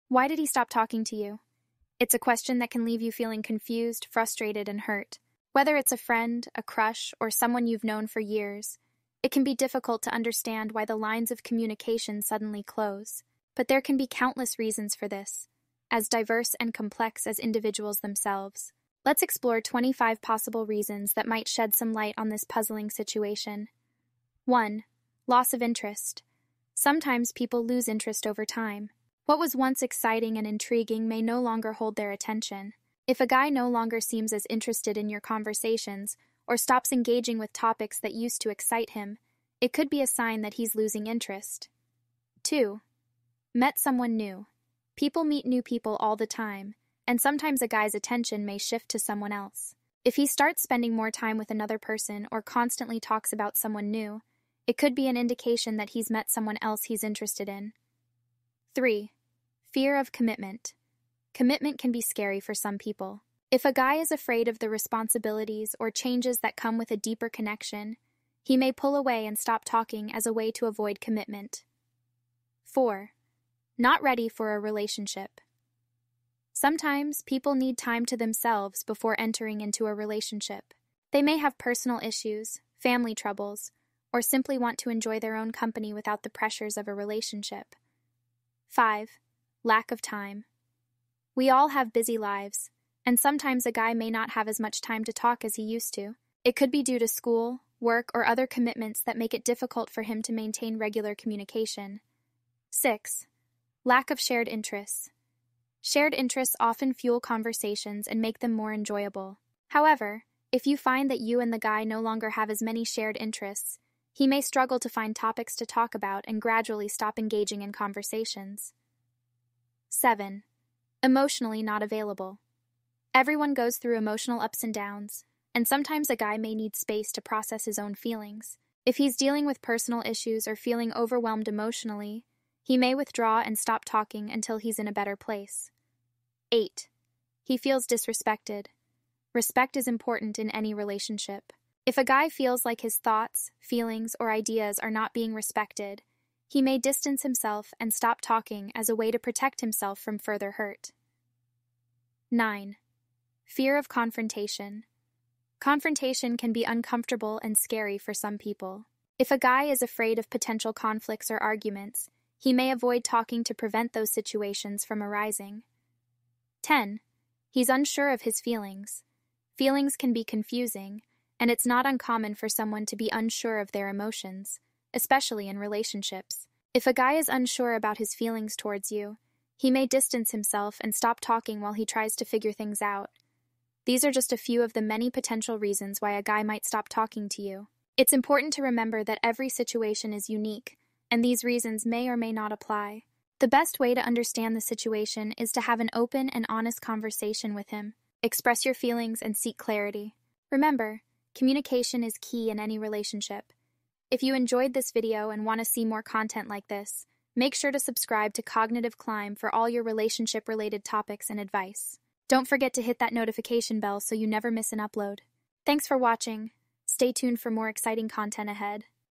The Shocking Reason Why He Suddenly Stopped Talking to You!
Why did he stop talking to you? If you've ever found yourself in this frustrating situation, you need to watch this video. Uncover the truth and discover the real reason behind his sudden silence. From mixed signals to commitment fears, we'll dive deep into the psychology of men and relationships to help you understand what went wrong. Don't miss out on this eye-opening discussion!